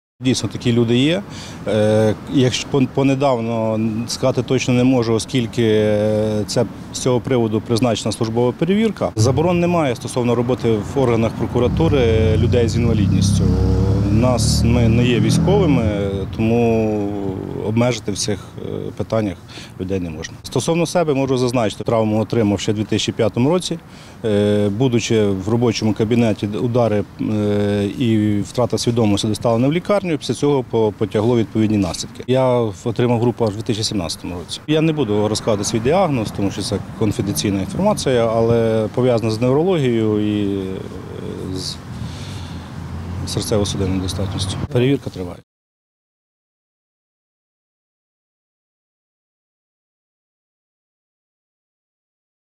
Керівник Хмельницької обласної прокуратури прокоментував інформацію про наявність інвалідності
Керівник Хмельницької обласної прокуратури Олексій Олійник розповів Суспільному: в структурі дійсно працюють люди з інвалідністю. За його словами, це не його заступники, а прокурори, які працюють у різних структурних підрозділах обласної прокуратури. Коли саме вони отримали групи інвалідності каже не уточнював.